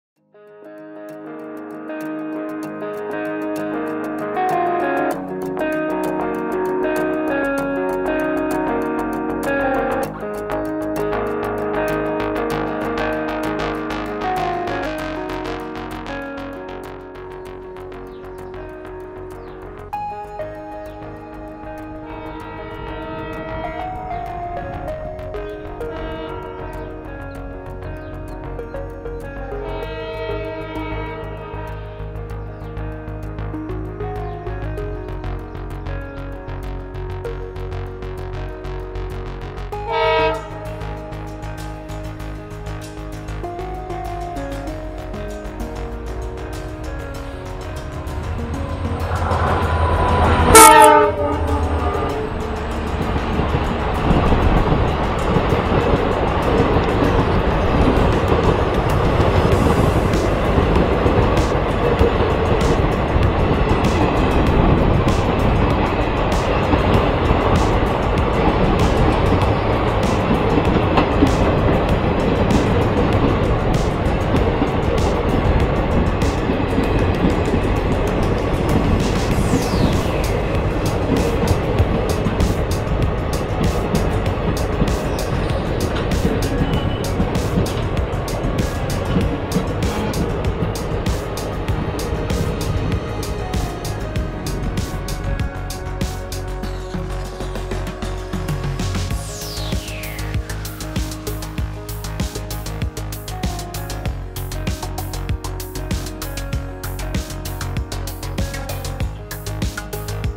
Karnataka sampark kranti express train
About Karnataka sampark kranti express
Sampark Kranti express of Karnataka is a train service in India, which runs between Yesvantpur near Bangalore to Hazrat Nizamuddin near Delhi. In addition another Karnataka Sampark Kranti started which runs between Chandigarh and Yeshvantapur. It provides a substitute to the Karnataka Express and also of superfast category. The frequency of these trains in compilation of all the Sampark Kranti is daily. These trains provide an alternative link between Bangalore City and New Delhi stations.

In late 2012, another longer bi-weekly train was introduced till Chandigarh via Delhi. The schedule of both the longer trains were synchronized from Yeshwantpur to Delhi. For some technical reasons the schedule of the trains was increased. Normally the train used to take 3 hours between Bhopal and Jhansi, whereas the trains now take 6 hours between them, which includes a whole night. The technical stops with the duration of the halt were also increased.

Location: On board
Camera: Nikon B500